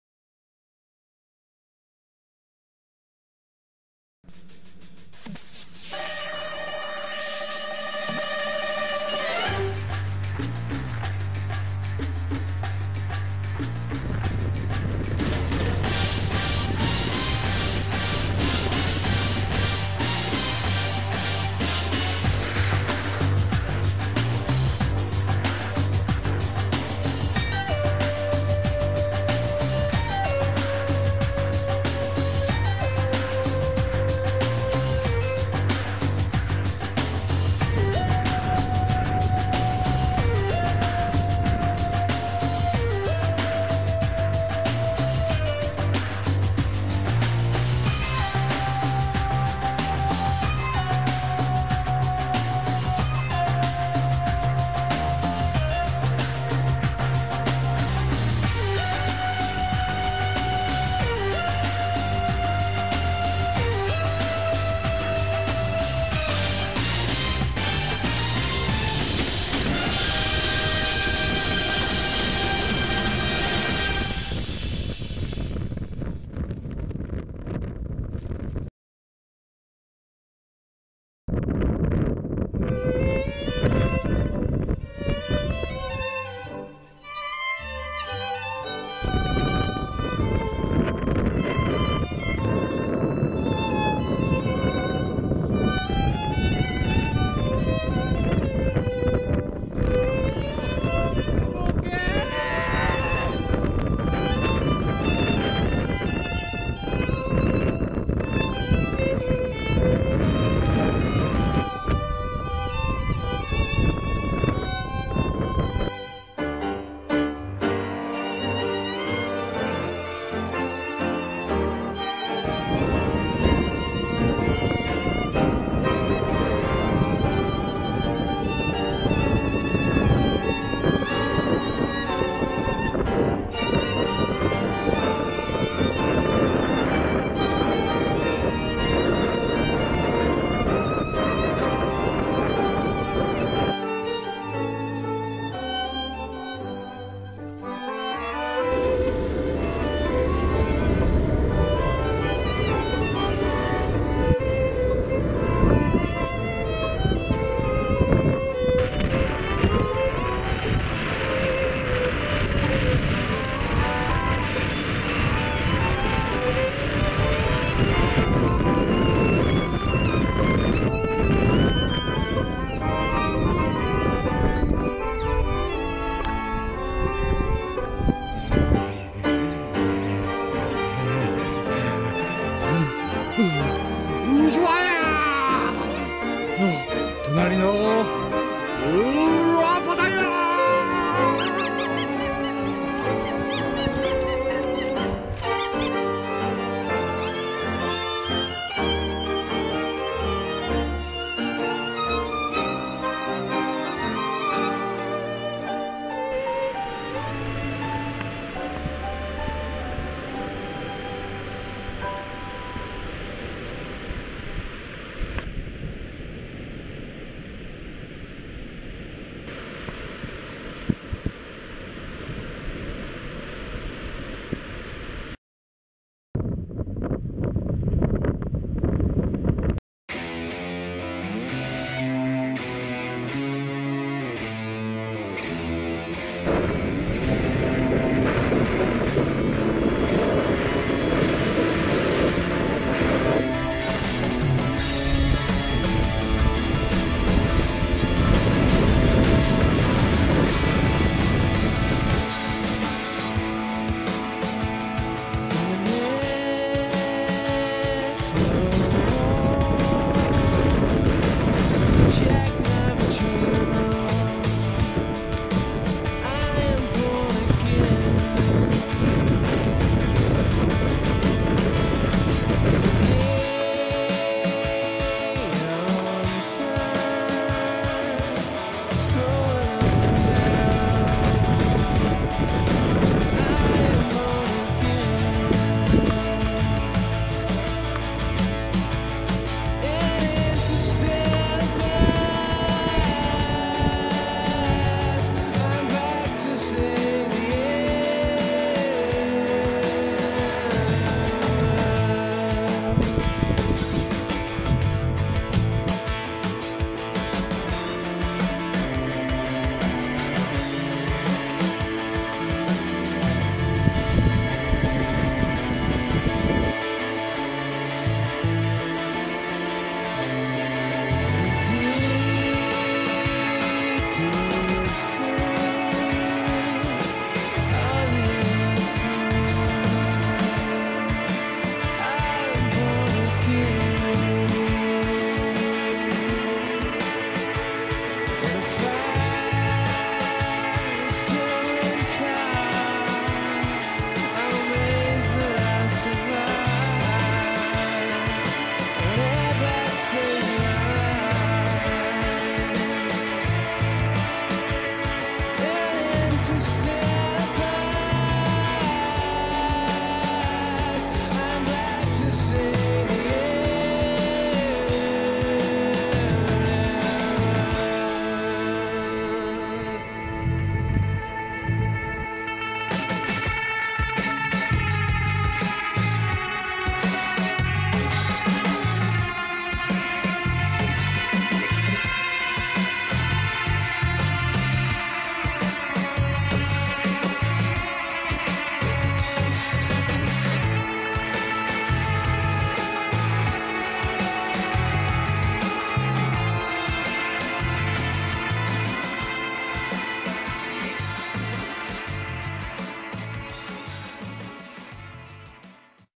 2014/11-2015/03 パタゴニア!V大作戦!! (JP Subtitle)
ブエノスアイレスから、爆風吹き荒れるパタゴニアを抜け、最南端ウシュワイヤへ!!そして、サンチャゴへと北上の道のりぃ!!